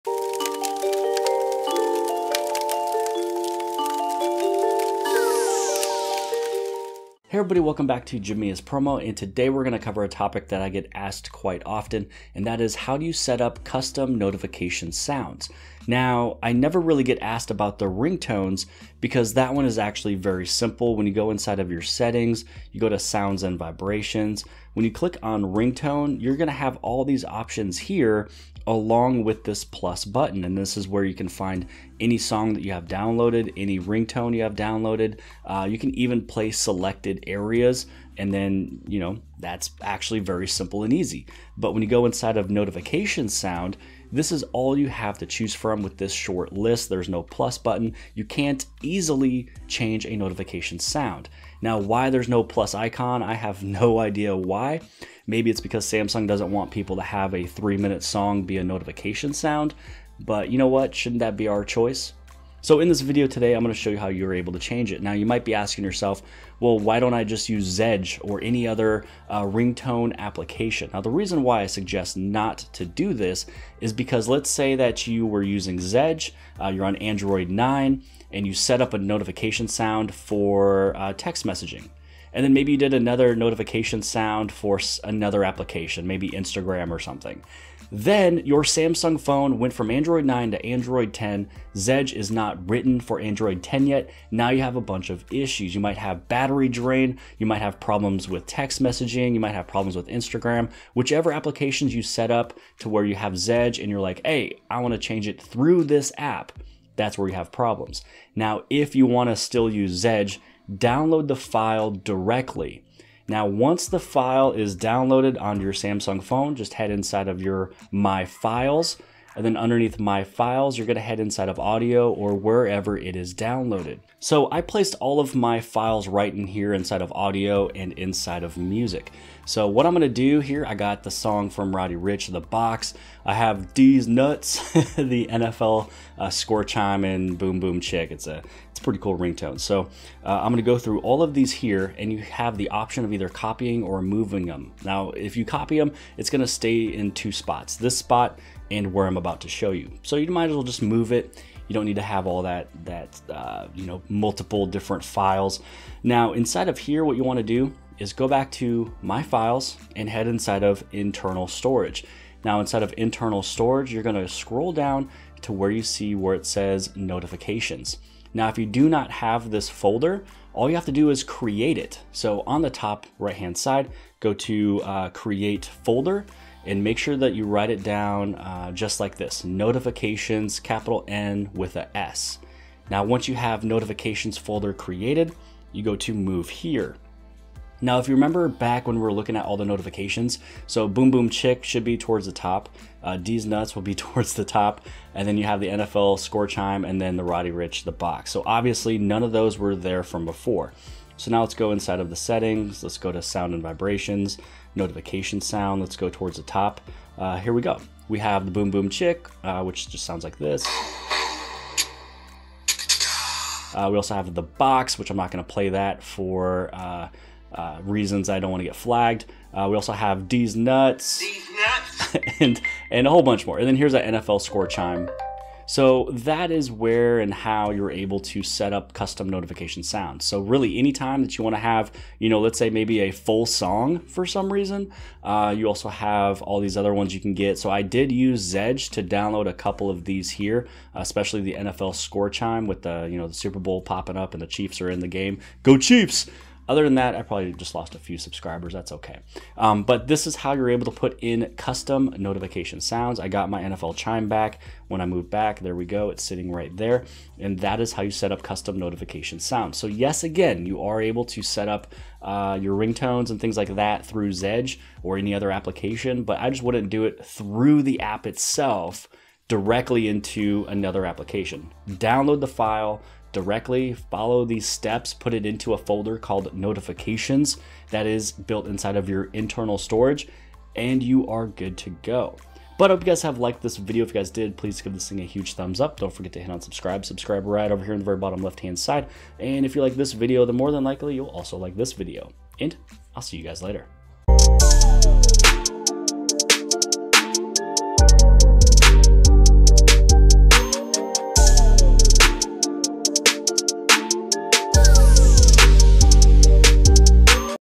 Custom Notification Sounds - The Proper Way!
Preorders Happening Now!

Welcome to the home of the best How-to guides for your Samsung Galaxy needs. In today's video, we will cover how to properly set custom notification sounds. Safe and without issues.